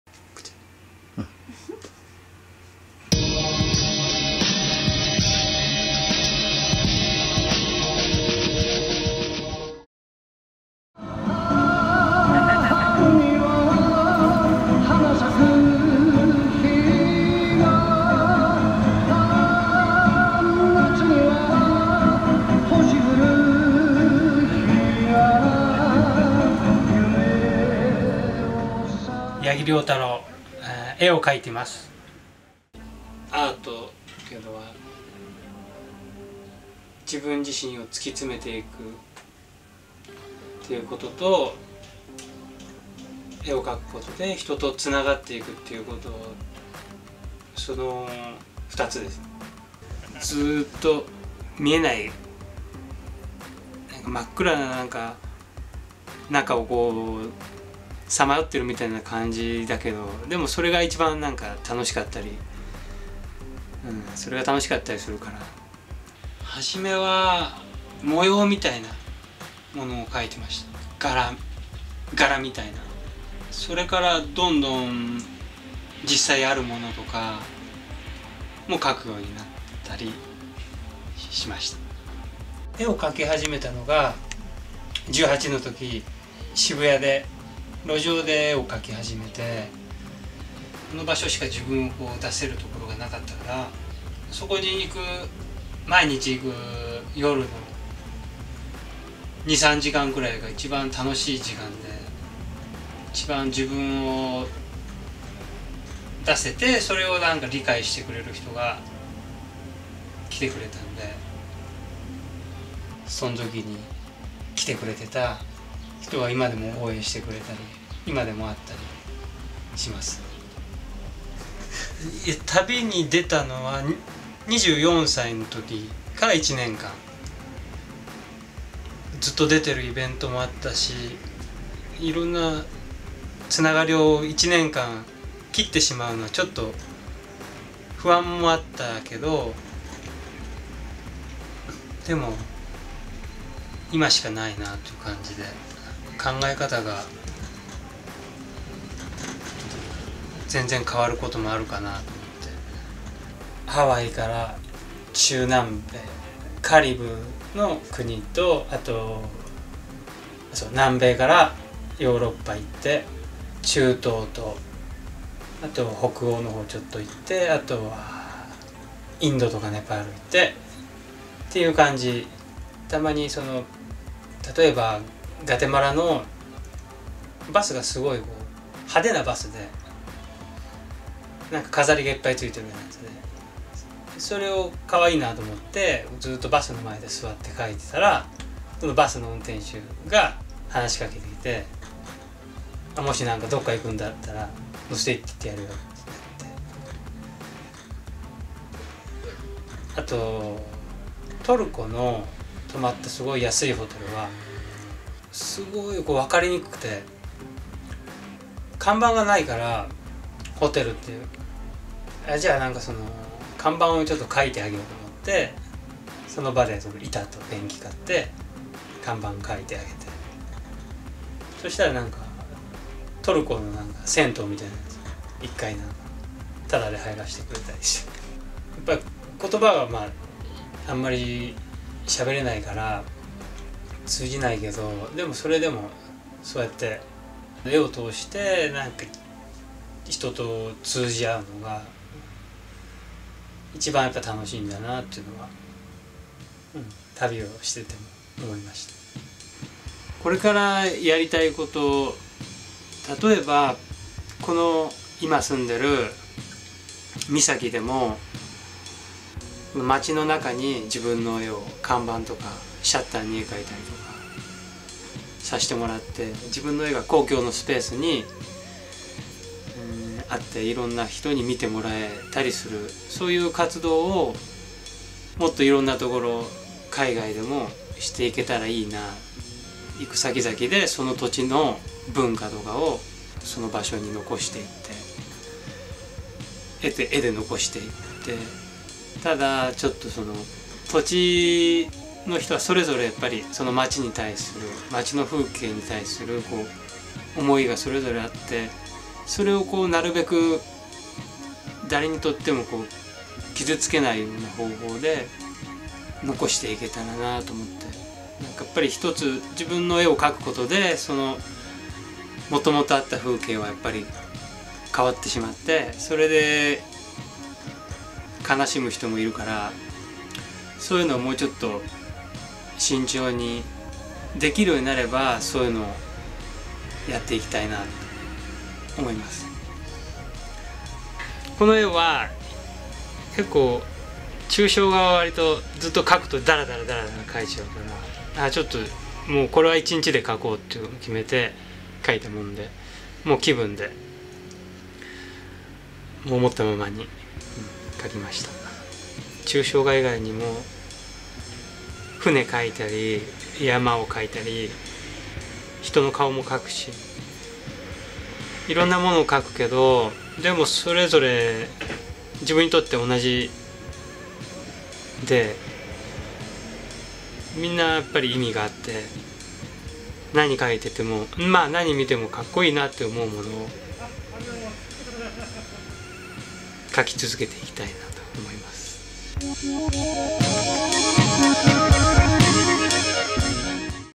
[StudioLifeTV #001] Meet Ryotaro Yagi a painter from Misaki Japan 画家やぎりょうたろう "人とつながるアート"
Studio Life TV #001 Ryotaro Yagi a painter form Misaki, Japan talks about his art and style, and his great journey with his art in many different countries.

神奈川の先端にある港町、三崎の画家やぎりょうたろう氏のスタジオへ訪問。彼のアートと人々へ対する想いを語ってくれました。

[StudioLifeTV #002] Nagamatsu Eiji a drummer from Yokohama ドラマー永松瑛二 "ドラムを追求すること。